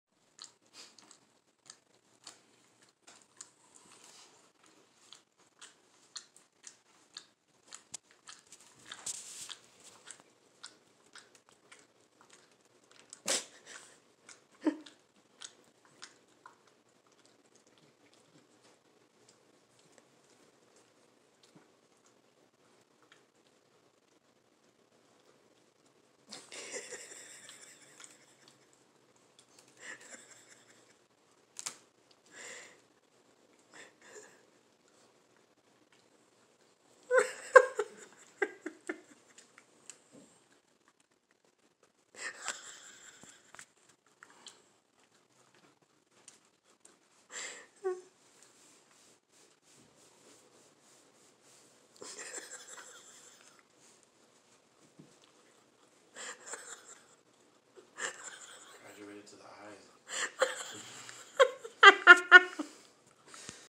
Border Terrier Grooming Love Fest
Cerri gives wee Charlie Girl a tongue bath and Charlie is so patient because she loves her big Sis.